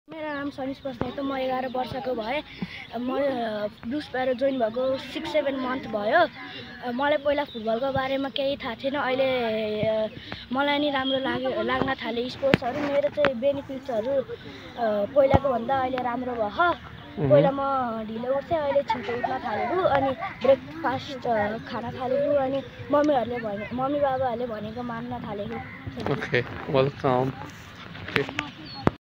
Sanish Basnet Feedback
Player feedback on what they changed and learned at Blue Sparrow Academy